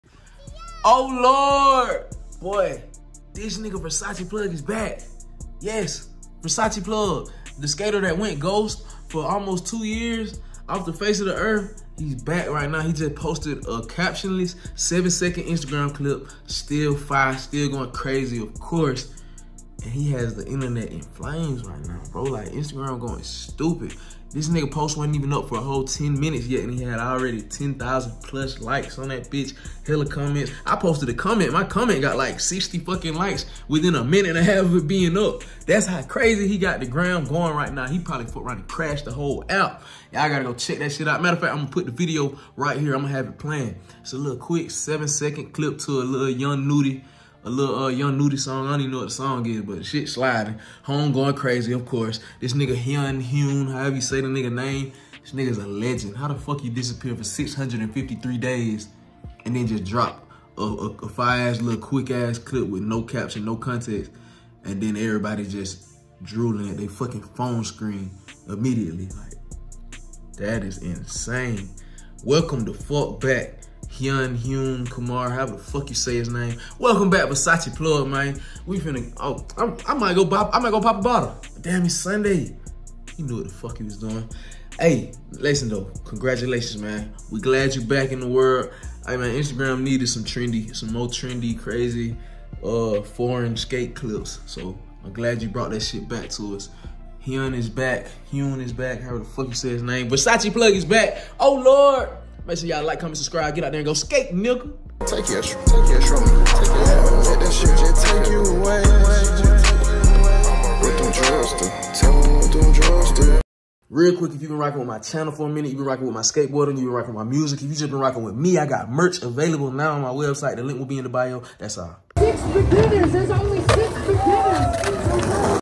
Versace Plug IS BACK !! 🤯🔥 .. (Hyun Kummer returns)
The man, the myth, the legend.. the PLUG is back on IG after a mysterious 653 day hiatus.

@LordDAndre on All Socials